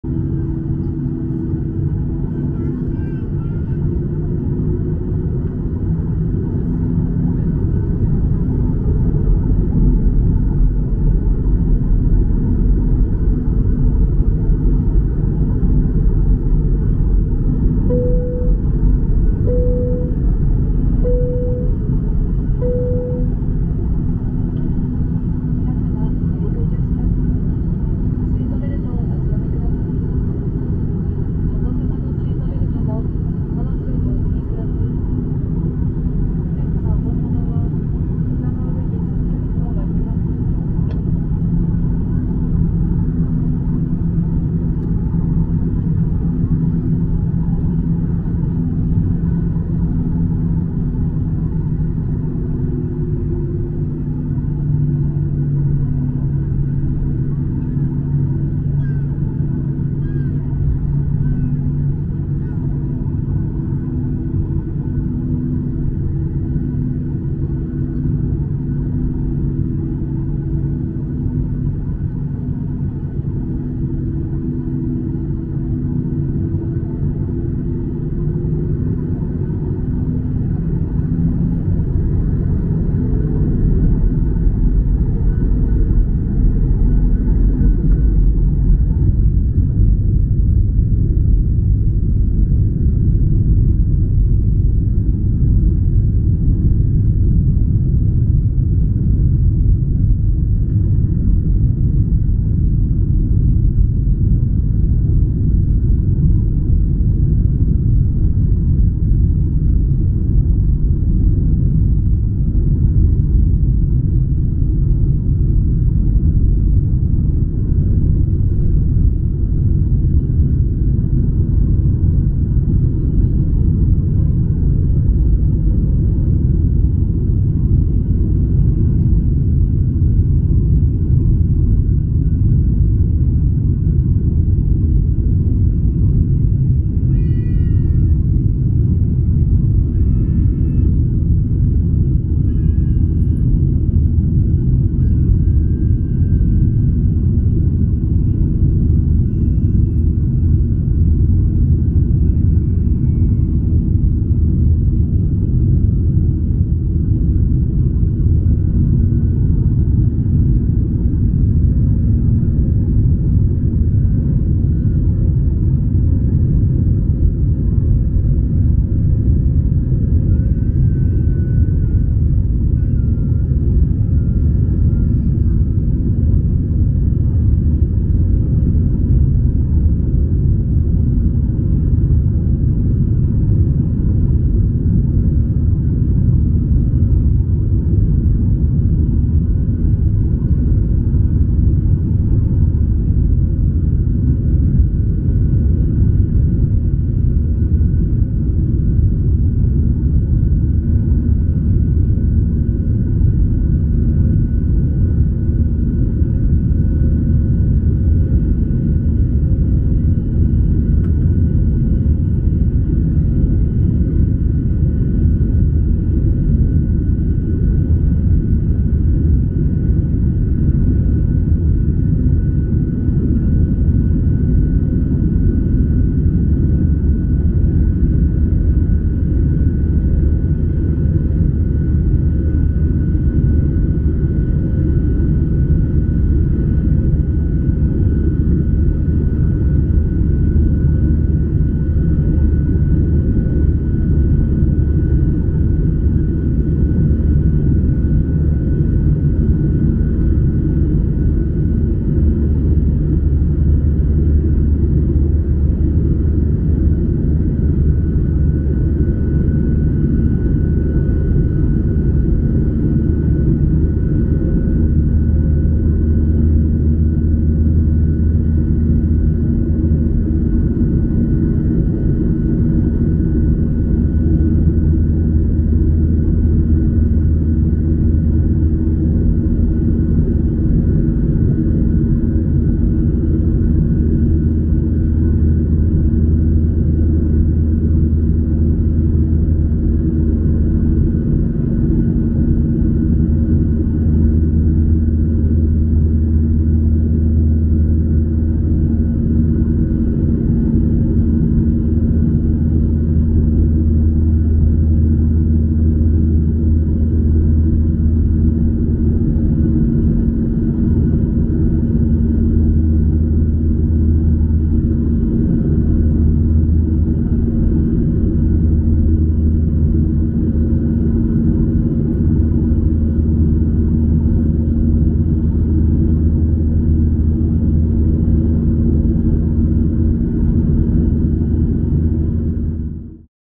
Taking off the Haneda Intl Airport, Tokyo in twilight
December 31 2022, I flew to Hokkaido to visit my old mother living alone there. My flight SKY723 took off Haneda Internationa Airport Tokyo at 1655 as schedued. And that was very the twilight time and beautiful scenery could be seen from side windows.
　　This video covers my flight taking off the air port and climbing up high in the sky, from where the mount "FUJI" silhouette backed by the sunset pale orange screen can be seen.
 　　I'd like you to enjoy it（╹◡╹）